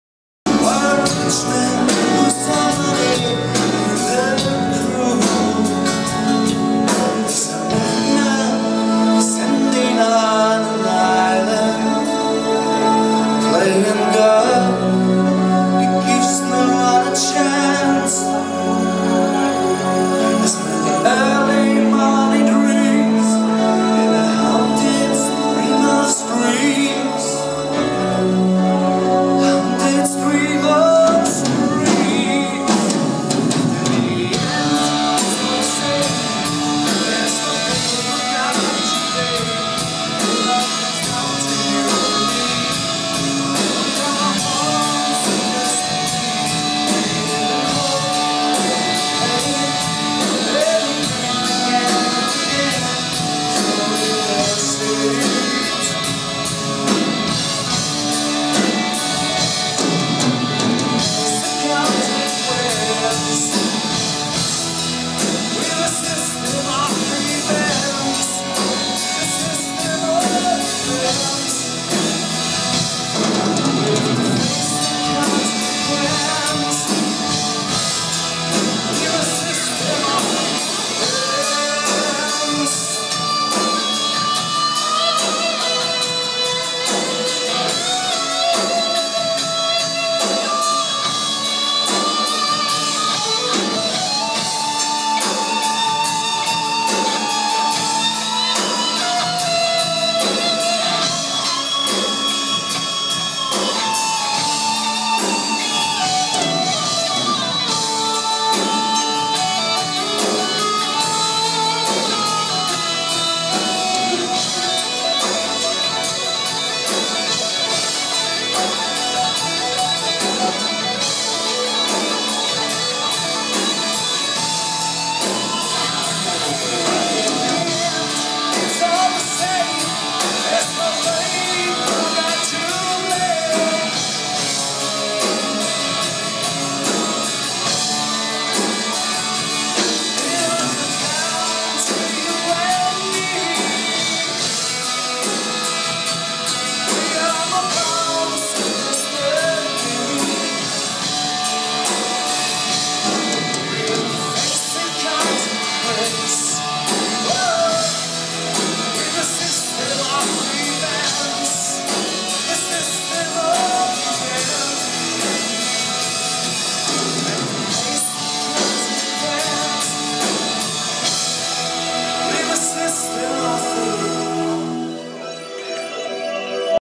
Crystal Palace
German prog-band Crystal Palace at Progdreams III, Culturpodium Boerderij Zoetermeer 4.4.2014.